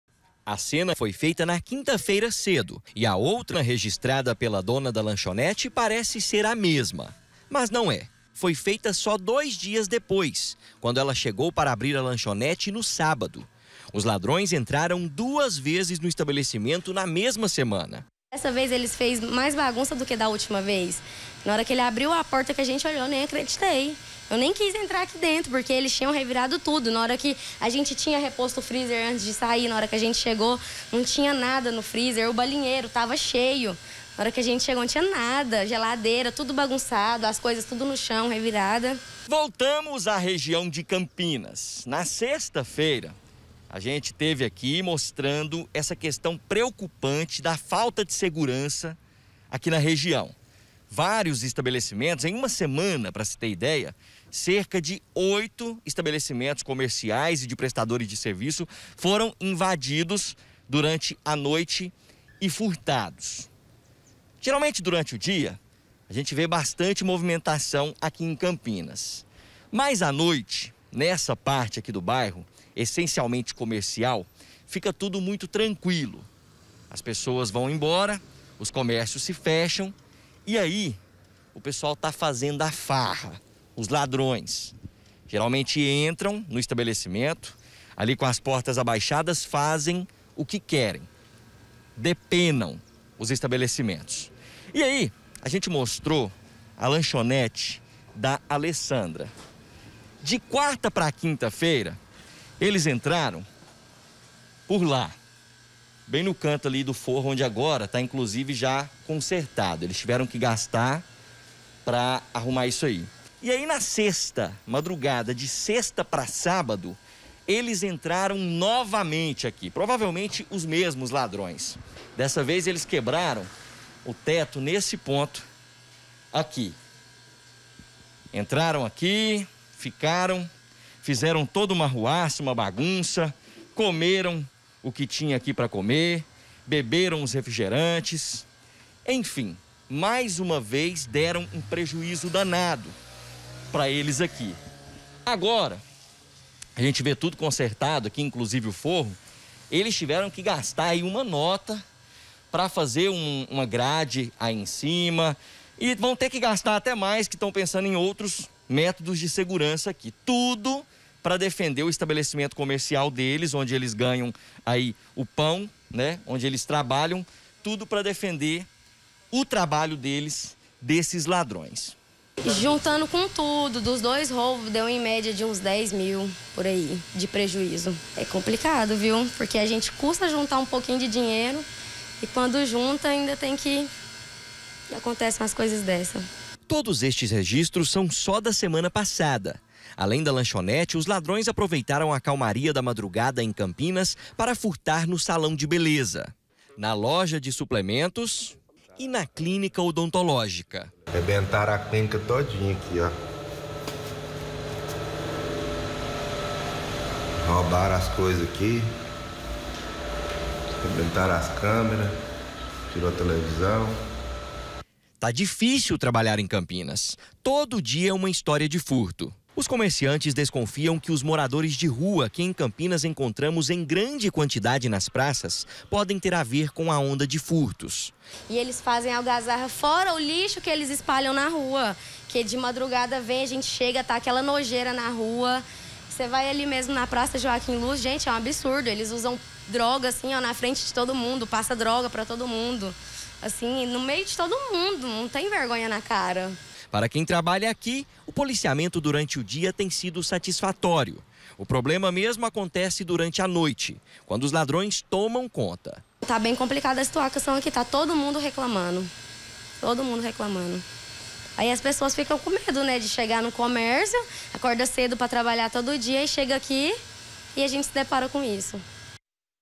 FURTOS EM CAMPINAS: OITO COMÉRCIOS SÃO INVADIDOS NO INTERVALO DE UMA SEMANA
COMERCIANTES DA REGIÃO DE CAMPINAS, AQUI NA CAPITAL, JÁ NÃO SABEM MAIS O QUE FAZER COM A BANDIDAGEM POR LÁ. CRIMINOSOS ESTÃO APROVEITANDO QUE O BAIRRO TEM POUCO MOVIMENTO DURANTE A NOITE E A MADRUGADA PARA INVADIR COMÉRCIOS E FURTAR OBJETOS E MERCADORIAS. QUEM SOFRE COM OS PREJUÍZOS PEDE SOCORRO!